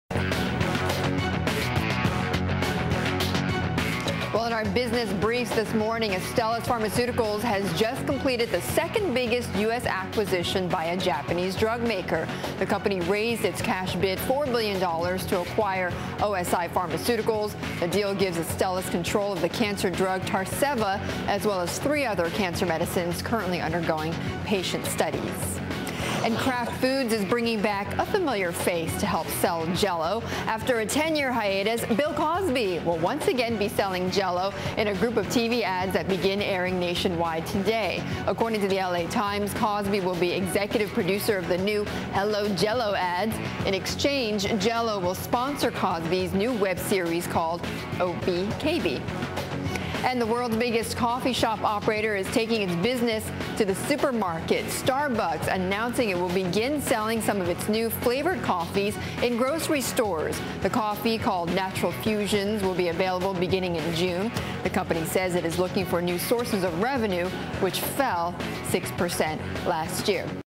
Astellas Raises Bid for OSI; Cosby Back Hawking Jell-O: Video
May 17 (Bloomberg) -- Bloomberg's Betty Liu reports on the latest breaking business news and top stories in today's Business Briefs. (Source: Bloomberg)